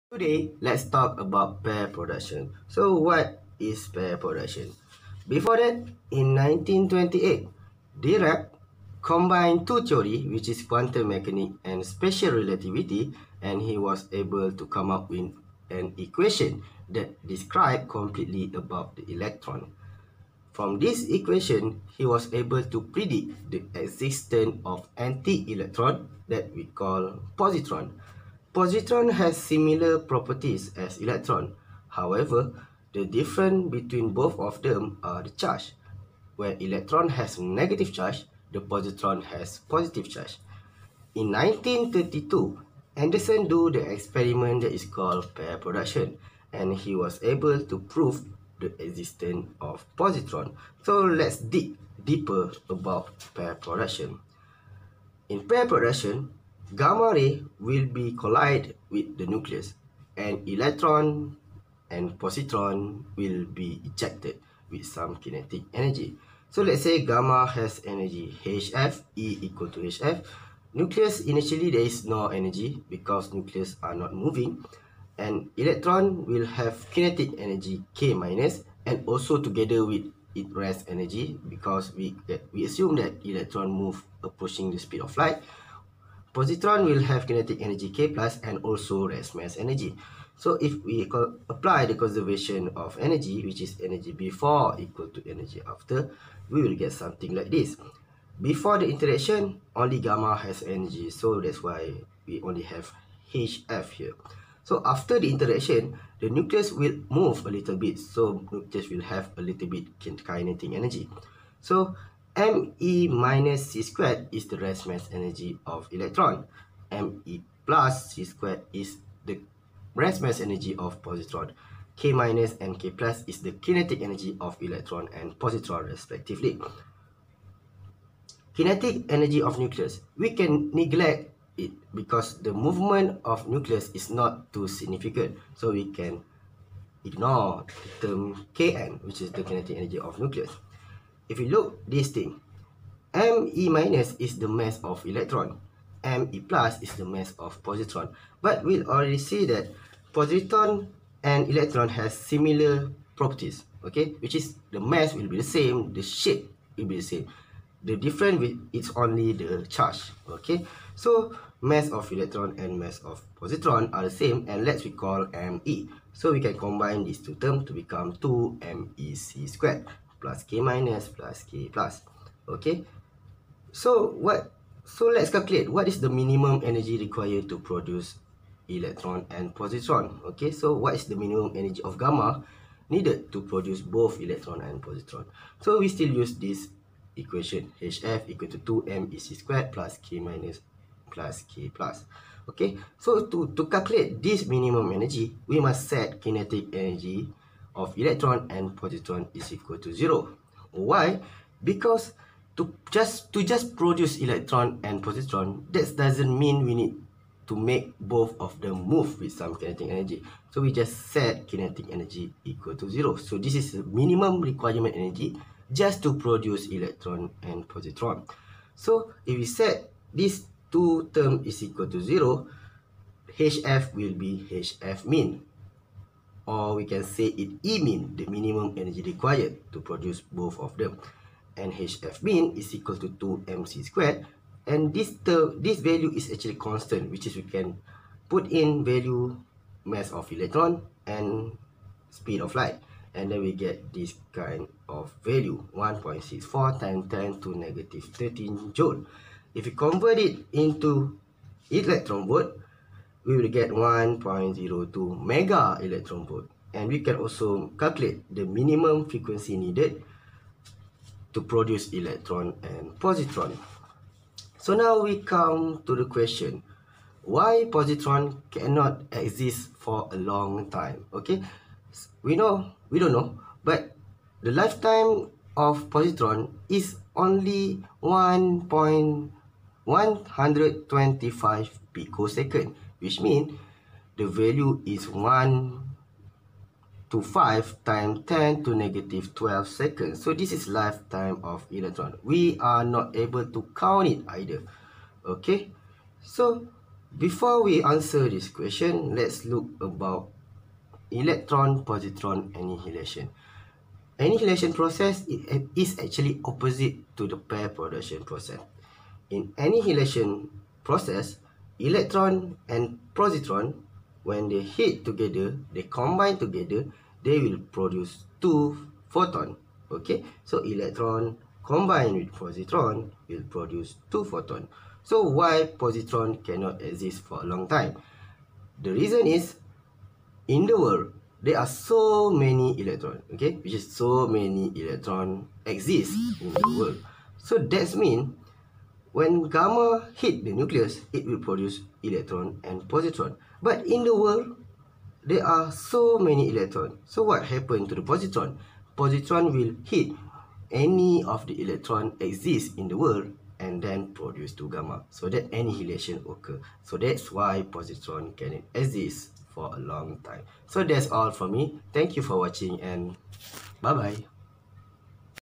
Pair Production
Any particle in the world has it own antiparticle. All matter will have antimatter. Even you may have antiyou.

All of this idea start from pair production experiment that proved the existence of antielectron.

So, enjoy this video and ENJOY PHYSICS!